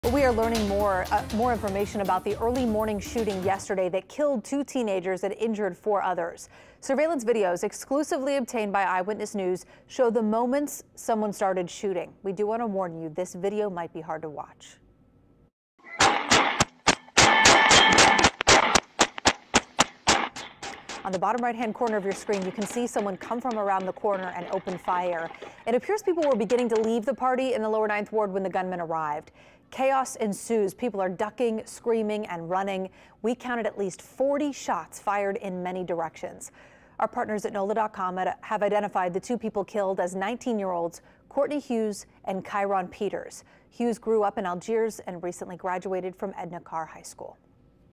Video captures deadly shooting at Lower 9th Ward party
The shooting happened during a party, in what neighbors believe to be a short-term rental property.